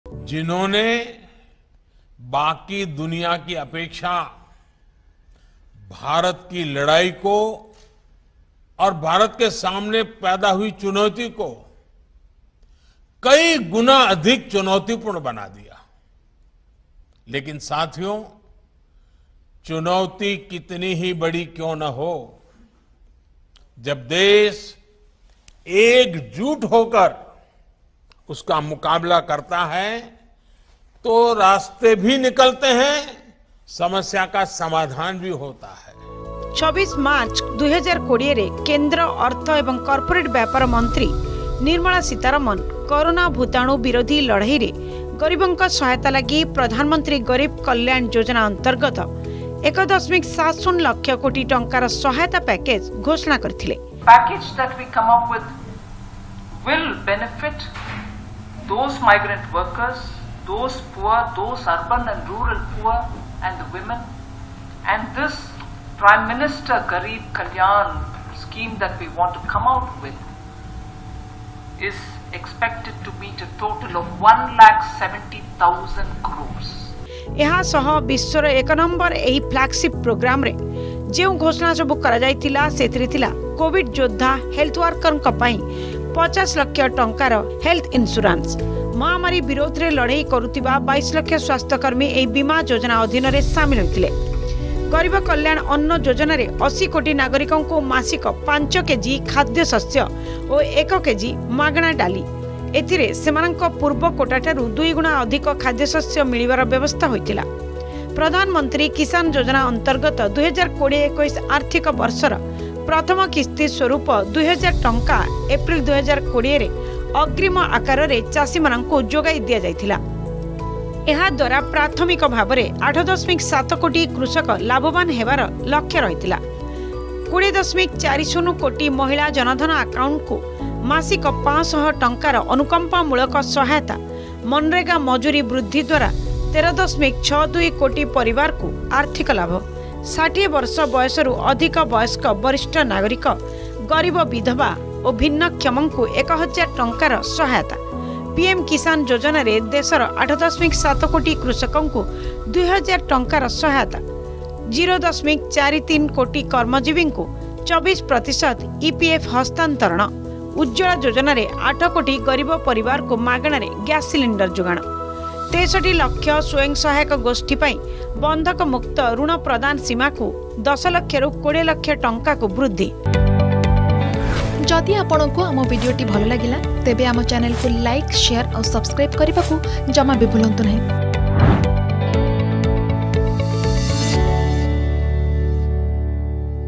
Loka Kalyan || 'ଦେଶ ଏକଜୁଟ୍‌ ହୋଇ ସମସ୍ୟାର ମୁକାବିଲା କଲେ ସମାଧାନର ବାଟ ଫିଟିଥାଏ'
ଦେଶ ଏକଜୁଟ୍‌ ହୋଇ ସମସ୍ୟାର ମୁକାବିଲା କଲେ ସମାଧାନର ବାଟ ଫିଟିଥାଏ : ପ୍ରଧାନମନ୍ତ୍ରୀ ମୋଦୀ